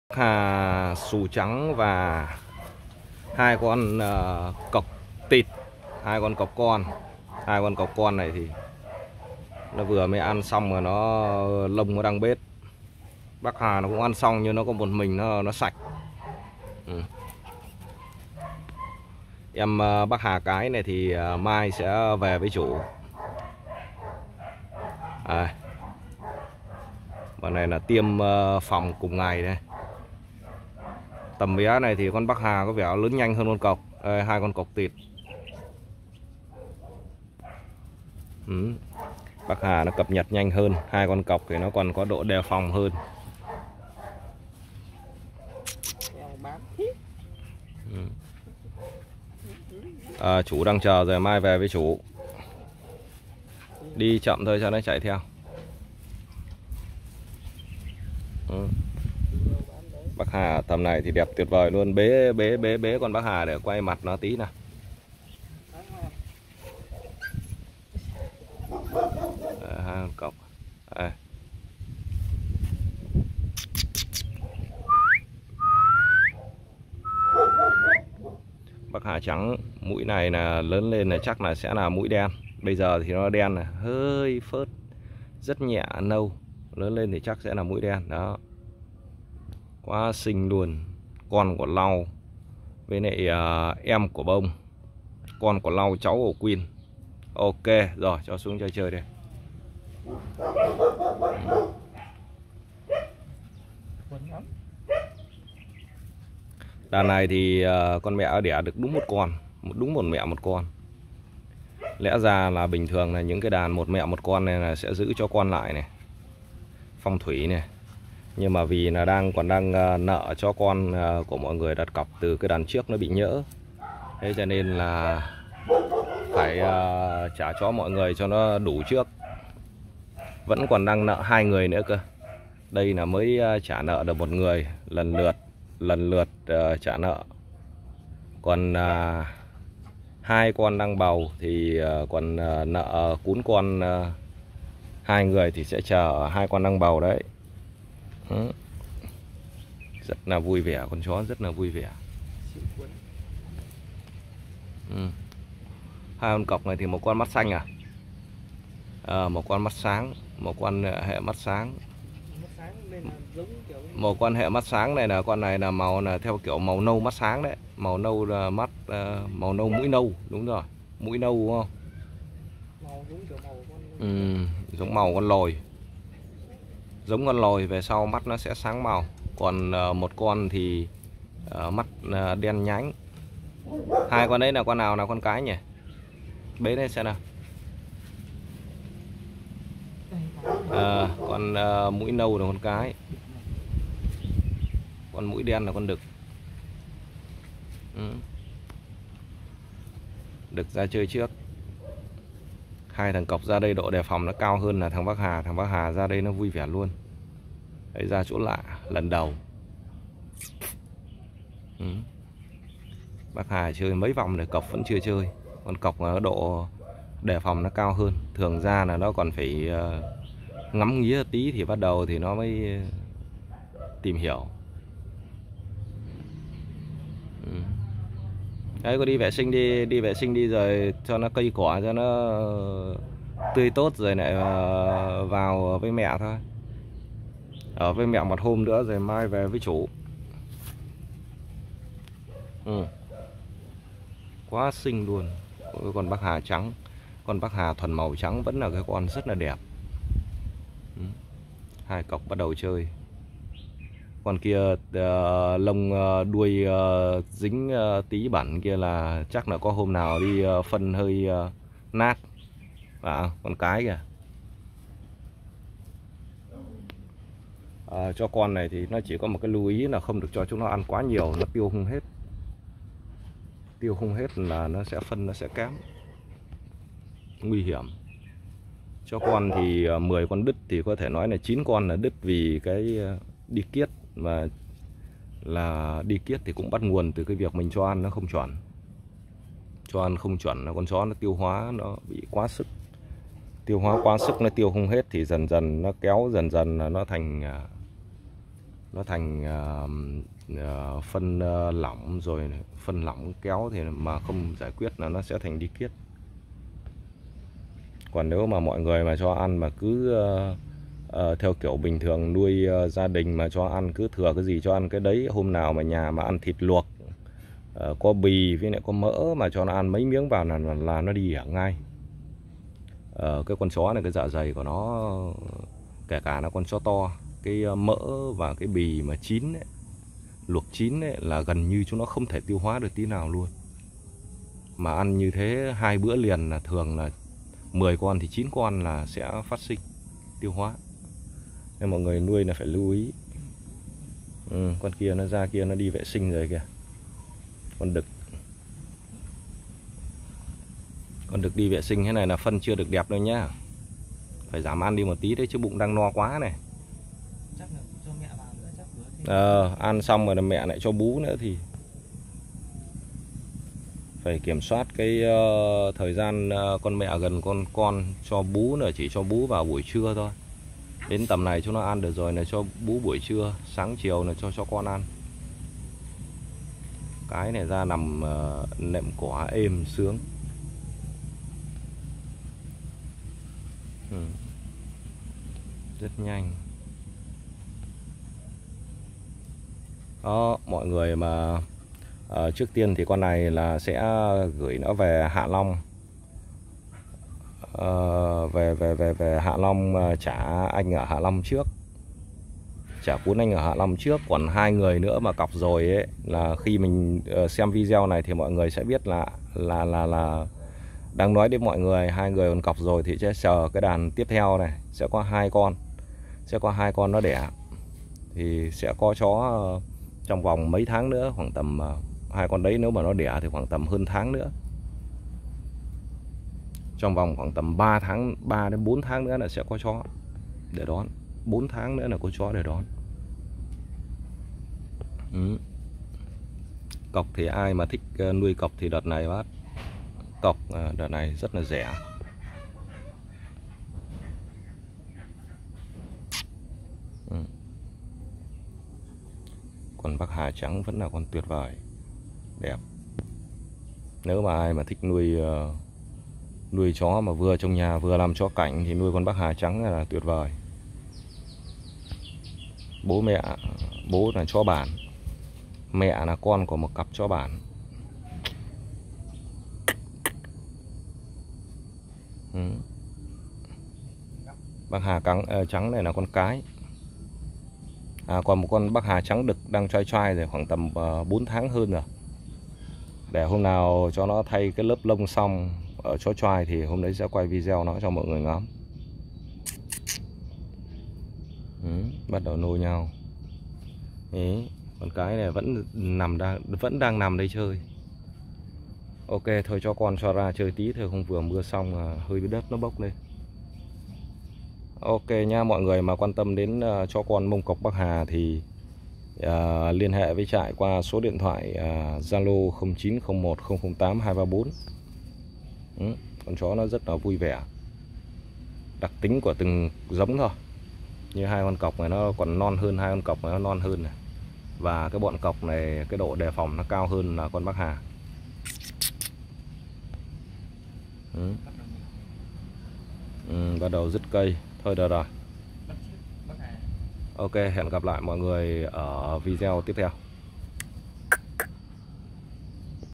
Chó con Bắc Hà xù trắng và mông cộc vàng giá rẻ| Puppy
Chó con Bắc Hà xù trắng và mông cộc vàng giá rẻ nuôi trông nhà. Hai giống chó ta bản địa to con, đẹp và rất thuần người thích hợp để nuôi gia đình. Chó con mông cộc đợt này giá rất rẻ mọi người nhé.